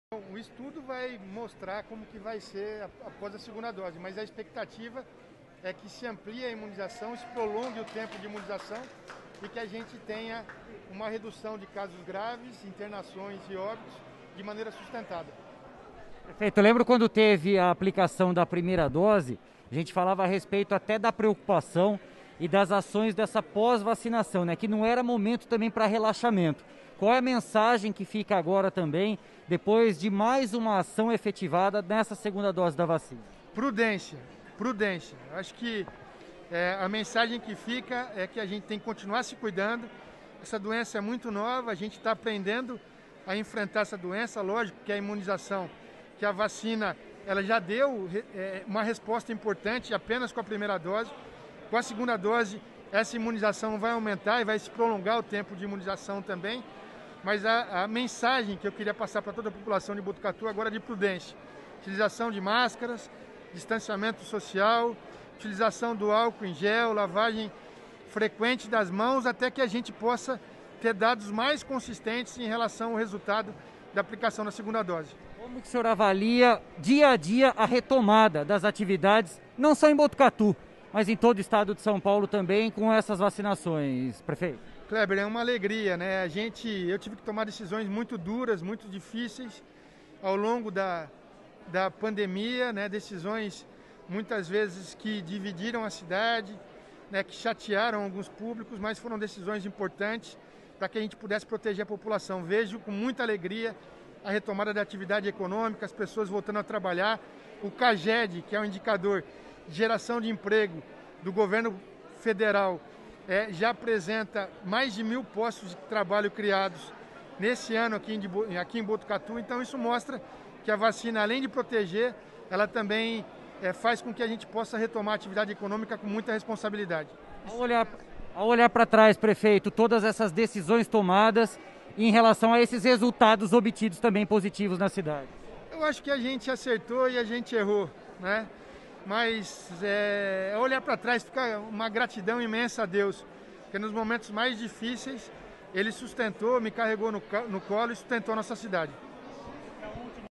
Prefeito sobre a vacinação em massa em Botucatu
Mario Pardini falou sobre o assunto à reportagem do 14news.